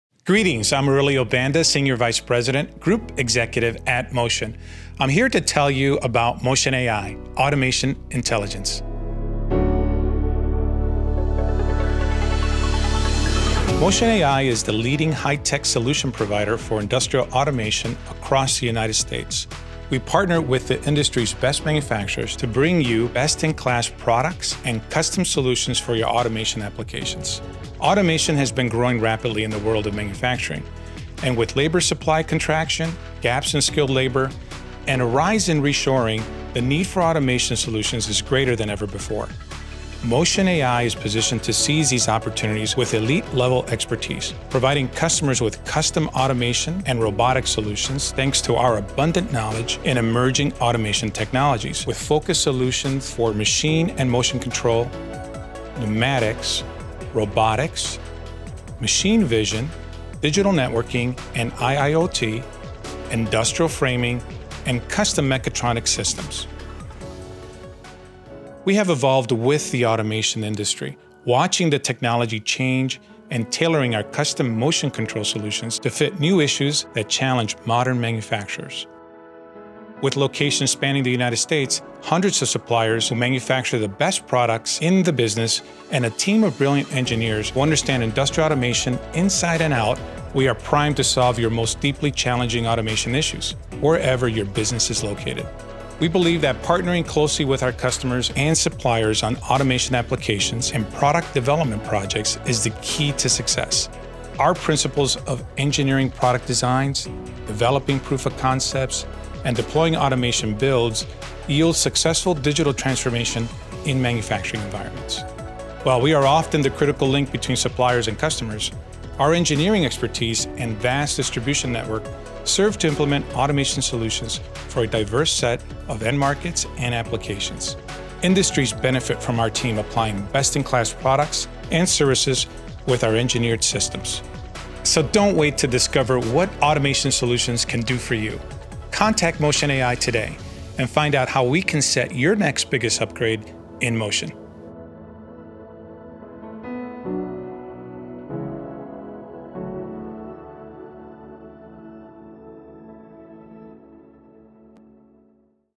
Discover Motion Ai: Automation Intelligence
Do you know what Motion Ai can do for you?

Motion Ai is the leading high-tech solution provider for industrial automation across the United States. We partner with the industry’s best manufacturers to bring you best-in-class products and custom solutions for your automation applications.

Automation has been growing rapidly in the world of manufacturing, and with labor supply contraction, gaps in skilled labor, and a rise in reshoring, the need for automation solutions is greater than ever before.

Motion Ai is positioned to seize these opportunities with elite-level expertise, providing customers with custom automation and robotic solutions, thanks to our abundant knowledge in emerging automation technologies, with focused solutions engineering for machine and motion control, pneumatics, robotics, machine vision, digital networking and IIoT, industrial framing and custom mechatronic systems.

We have evolved with the automation industry, watching the technology change and tailoring our custom motion control solutions to fit new issues that challenge modern manufacturers.

With locations spanning the United States, hundreds of suppliers who manufacture the best products in the business, and a team of brilliant engineers who understand industrial automation inside and out, we are primed to solve your most deeply challenging automation issues, wherever your business is located.

We believe that partnering closely with our customers and suppliers on automation applications and product development projects is the key to success. Our principles of engineering product designs, developing proofs of concept, and deploying automation builds yield successful digital transformation in manufacturing environments.

While we are often the critical link between suppliers and customers, our engineering expertise and vast distribution network serve to implement automation solutions for a diverse set of end markets and applications. Industries benefit from our team applying best-in-class products and services with our engineered systems.